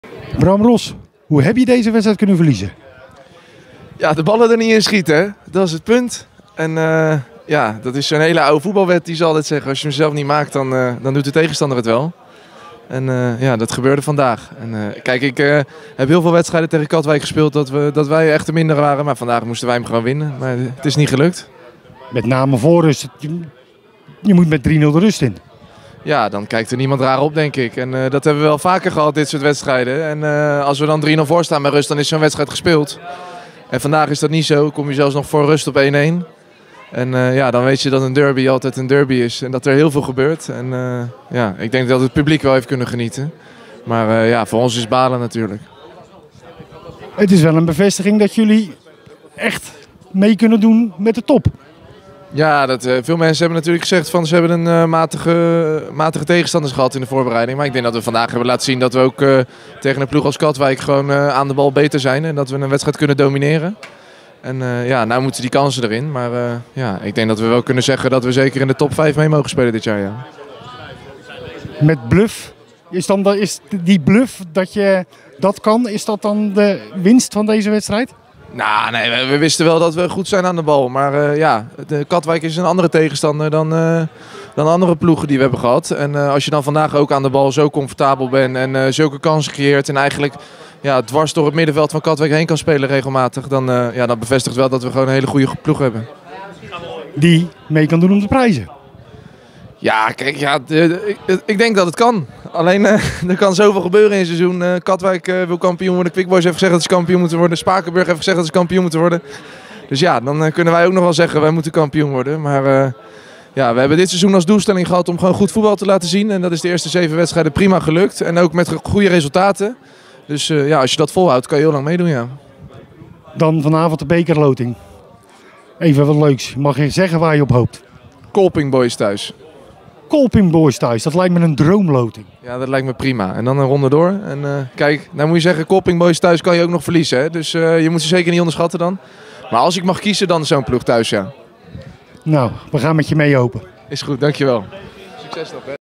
Ros: ‘Het publiek heeft kunnen genieten, voor ons is het balen’
Bram Ros verloor zaterdag met Rijnsburgse Boys van Katwijk (1-2), een wedstrijd die volgens de Rijnsburgse captain eigenlijk een andere uitkomst hadden moeten krijgen. De verdediger baalde, maar haalde wel vertrouwen uit het vertoonde spel.